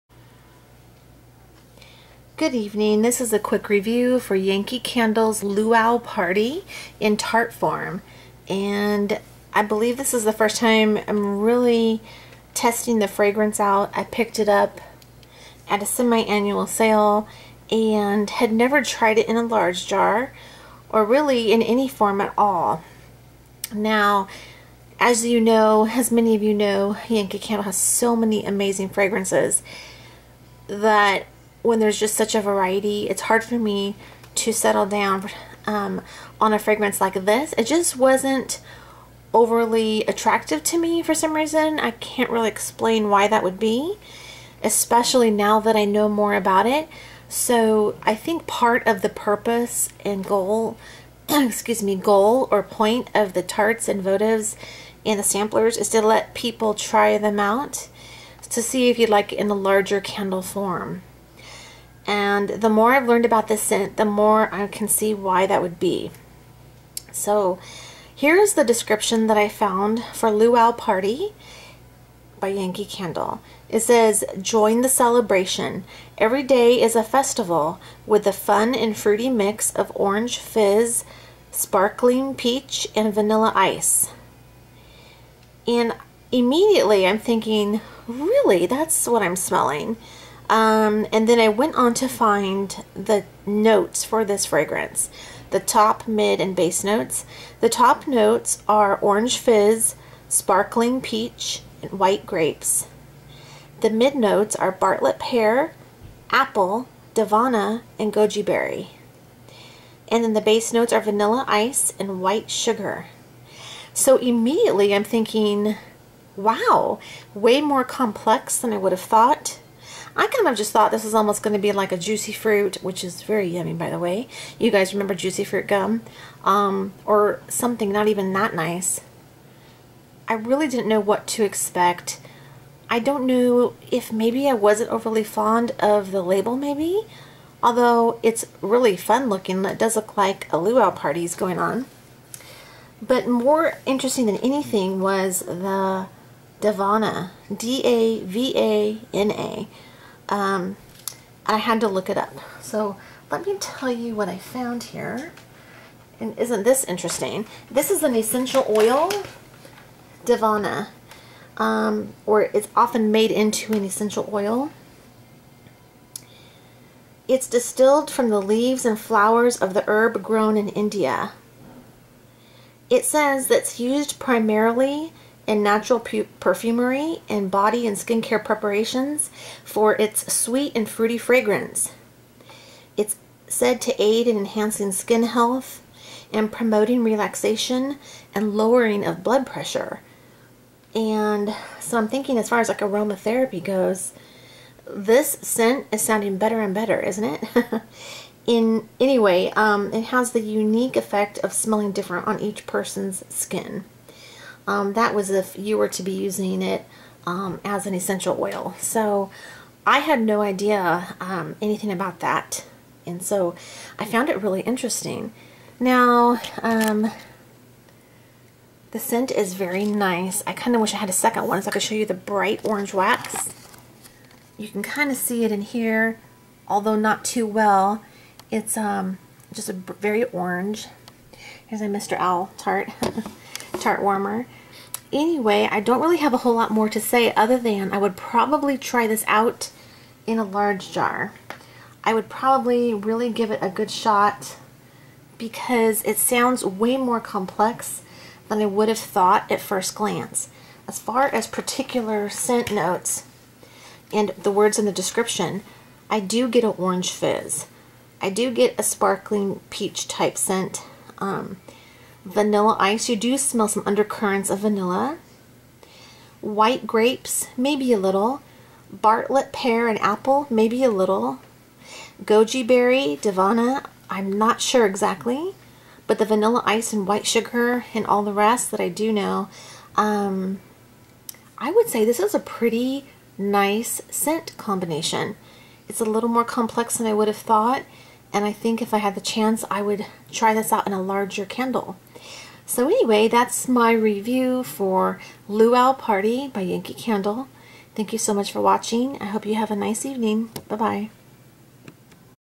Luau Party by Yankee Candle - A Review of the Tart
Found this a while back, and upon melting, I found myself rather surprised!!